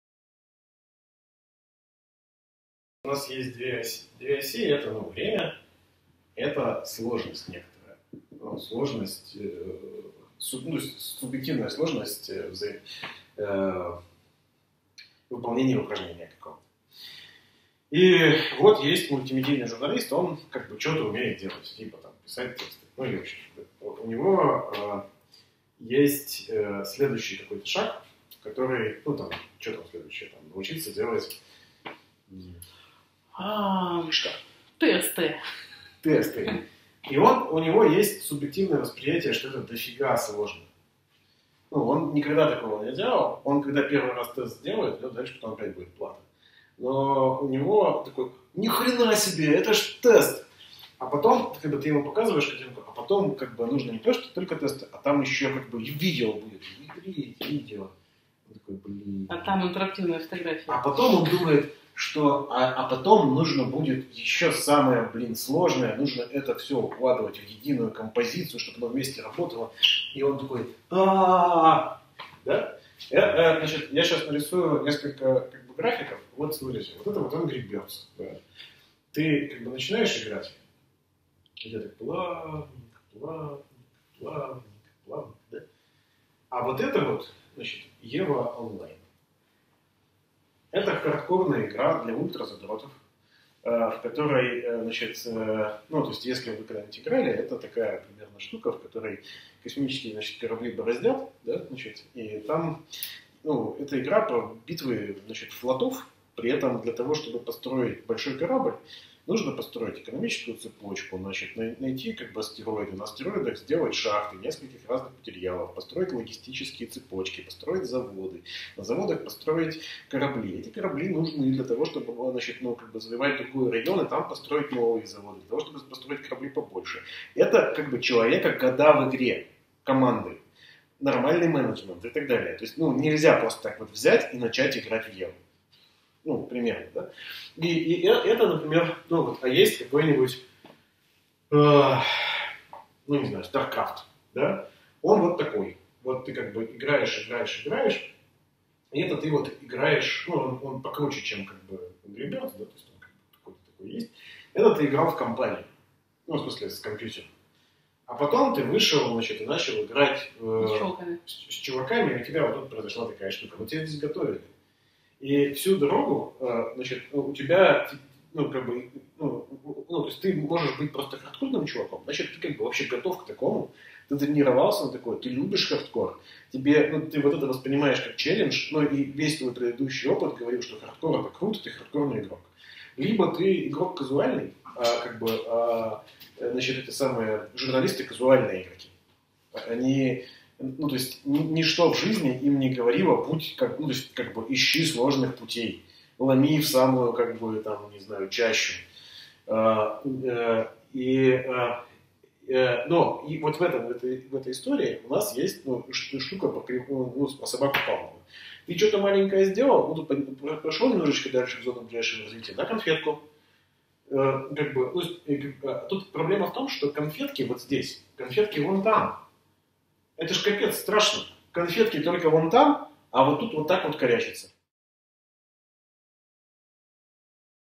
Алексей Кулаков, Ridero: "Журналисты - казуальные игроки"
Интервью Оксаны Силантьевой с Алексеем Кулаковым 16 июня 2016 года, Екатеринбург